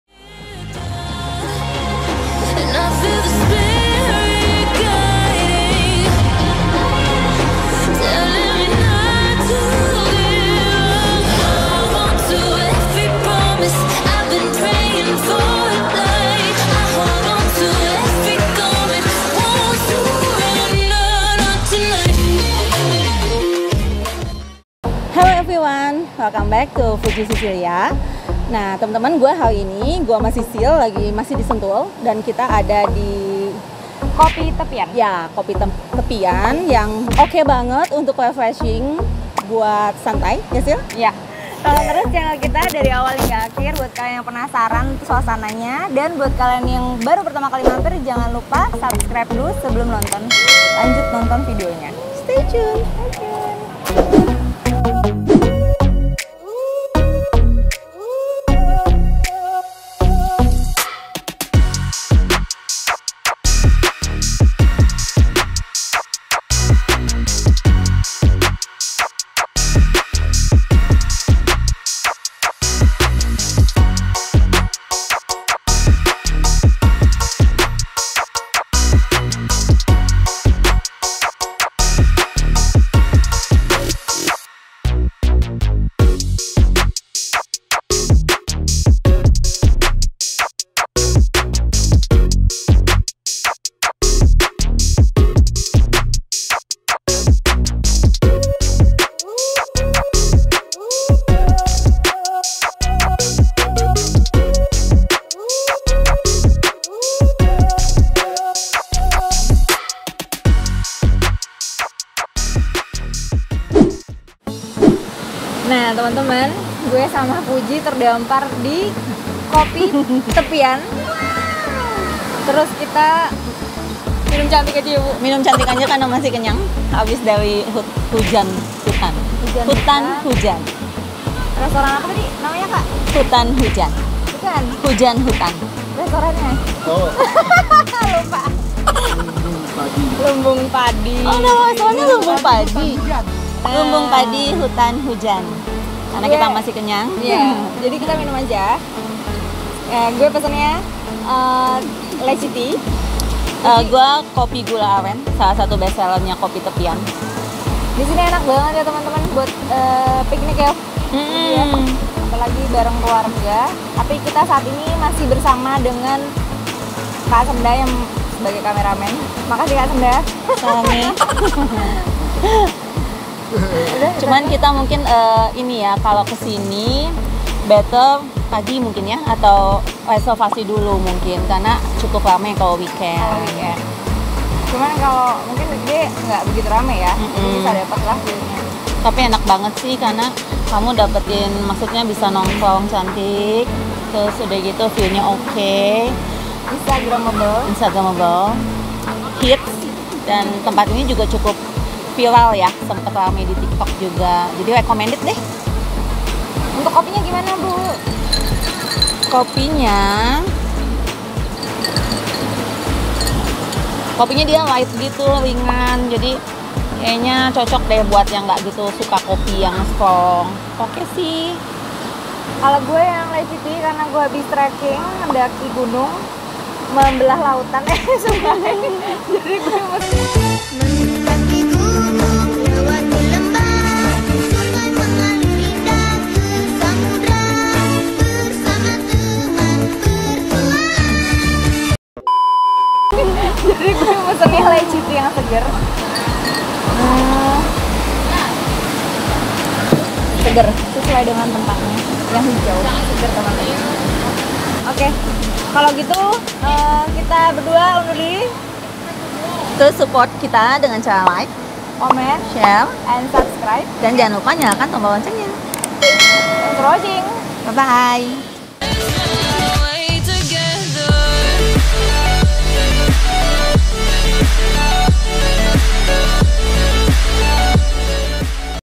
TEMPAT NONGKRONG DI BOGOR TERBARU PEMANDANGAN ALAM HITS VIRAL REVIEW & RUTE JALAN KOPI TEPIAN SENTUL
Tempat nongkrong di Bogor terbaru pemandangan alam hits viral | Review dan rute jalan kopi tepian sentul. Bisa nongkrong sambil main di sungai, ada camping ground dengan view sawah dan sungai. seru banget. Review lengkap cafe baru di Sentul termasuk review harga dan menu. Tempat nya hits banget, river view. Asik buat ngopi kece.

Lokasi : Kampung Babakan Ngantay RT 02/RW 08 Karang Tengah, Sentul, Kec. Babakan Madang, Kabupaten Bogor, Jawa Barat
Jam buka :
Senin, Rabu, Kamis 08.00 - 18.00
Jumat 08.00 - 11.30 ; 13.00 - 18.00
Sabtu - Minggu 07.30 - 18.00
Selasa : TUTUP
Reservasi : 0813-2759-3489

Happy watching!! 🥰